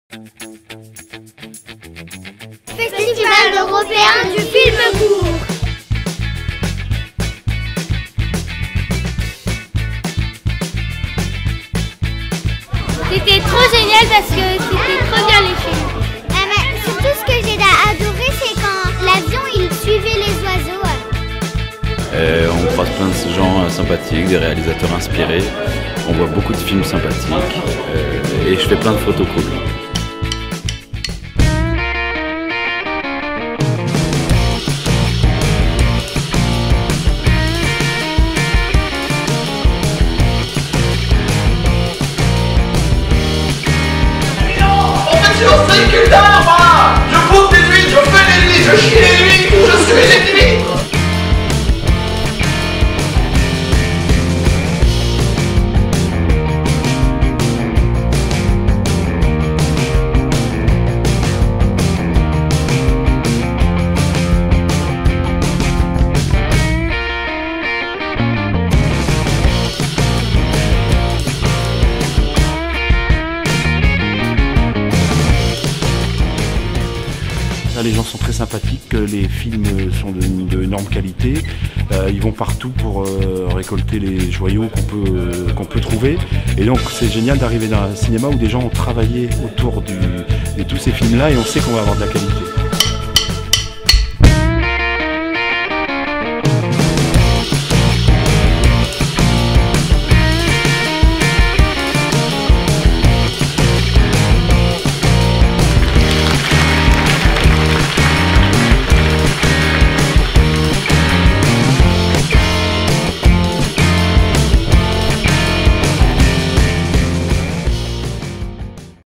Festival Européen du film court de Brest
Voici une petite vidéo que nous avons faite sur le festival Européen du film court de Brest. C'est le deuxième festival de court métrage en France et en Europe. Il à été créé en 1986 et joue un rôle majeur dans la vie culturelle à Brest, en proposant un temps fort de cinéma chaque année en novembre au Quartz, au Multiplexe Liberté, aux Capucins ainsi qu’au cinéma Les Studios. Bon visionnage ! ;)